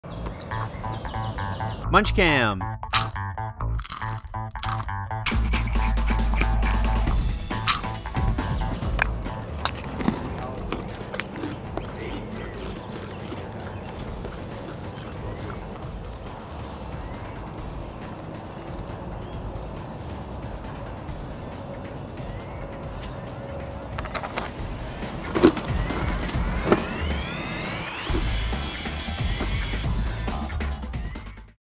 Munch Cam - Lightning the Donkey
Whether museum animals chomp, chew, gnaw, or nibble their food, they all munch! Now you can get a unique, close up and personal perspective on the different ways our Museum animals enjoy a meal with Munch Cam.